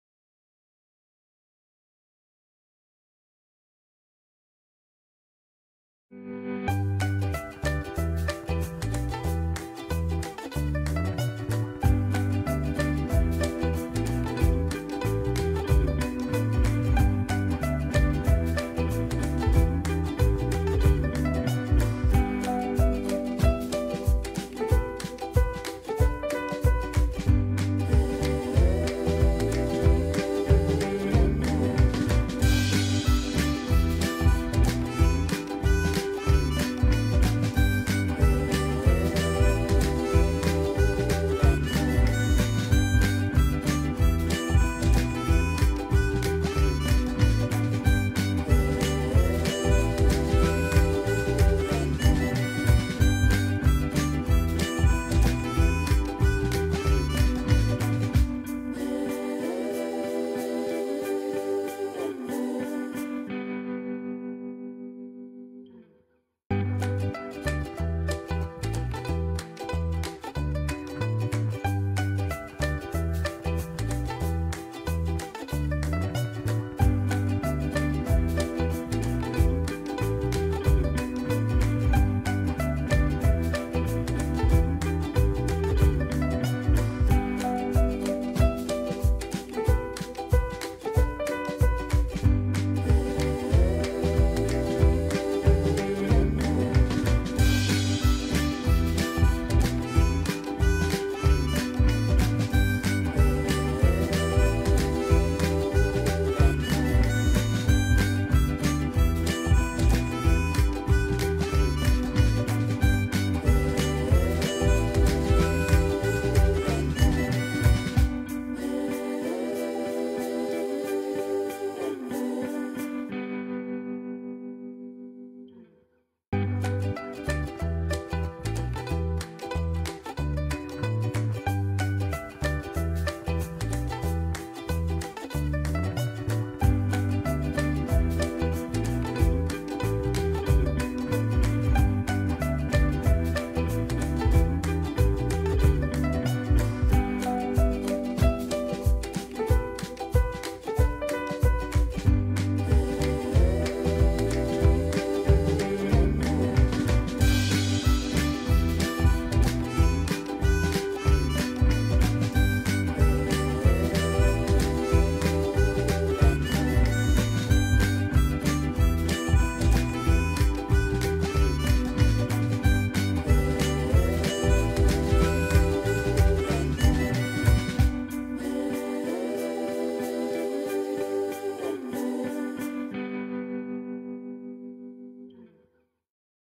Holi - Festival of Colors Rangoli Design
Rangoli is an art form in India consisting of making designs on the floor using colored sand, petals, flour or rice. It is an ancient tradition celebrated during festivals.